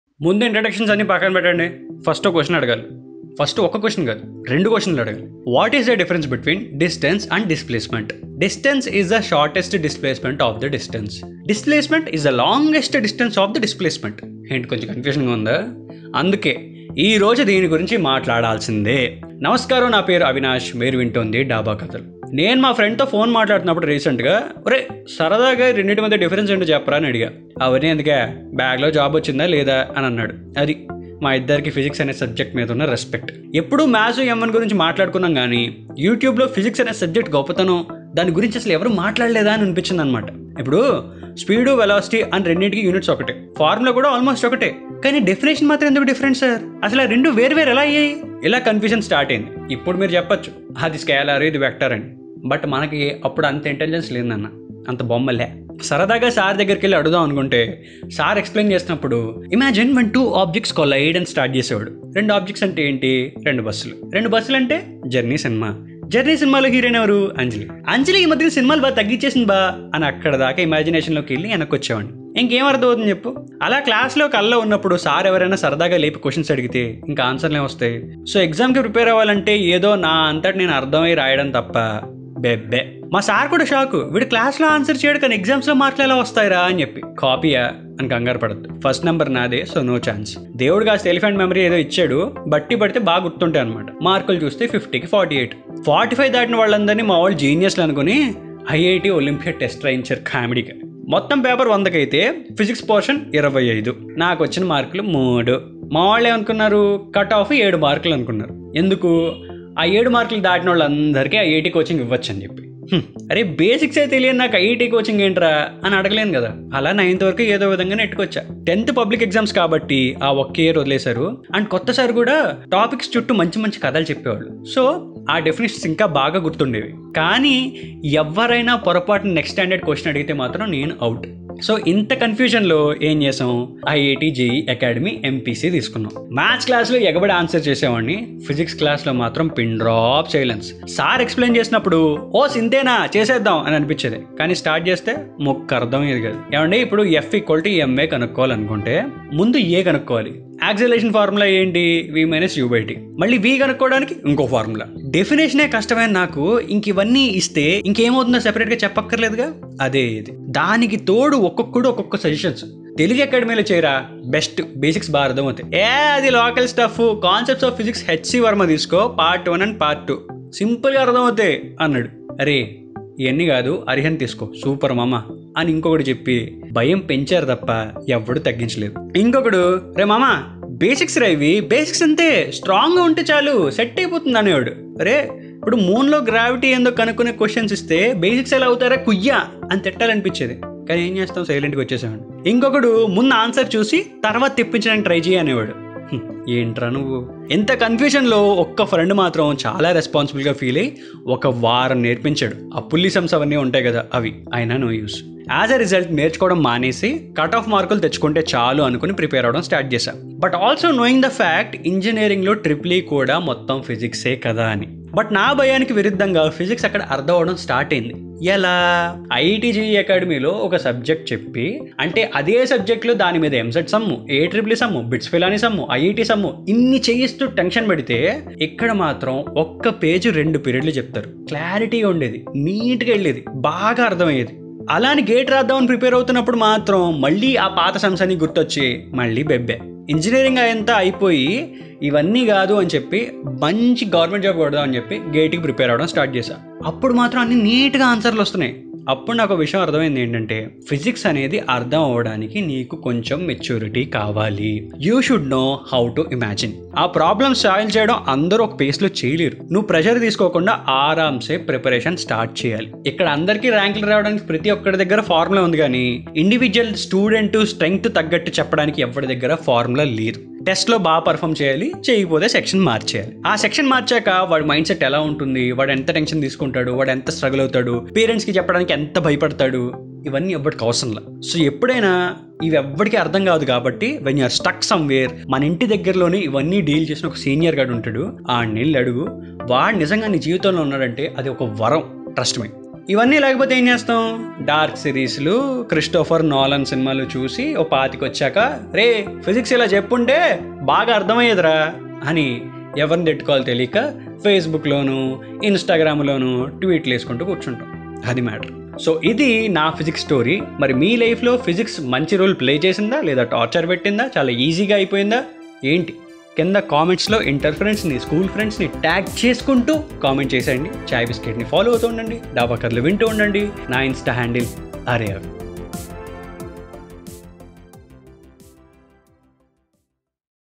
డాబా కథలు! A Telugu Podcast by Avi | E81 - Physical Distance | Chai Bisket Stories
Corona Times ki dooramga, mana chinnappati kathalloki veldhaama koddisepu ? Idi vinte Corona taggademo kaani tension aithe taggutundi.
Hit the play button I say.

డాబా కథలు!
A Podcast by Millennial which connects the memories with experiences.
Episode 81 - Physical Distance
Every day @7pm.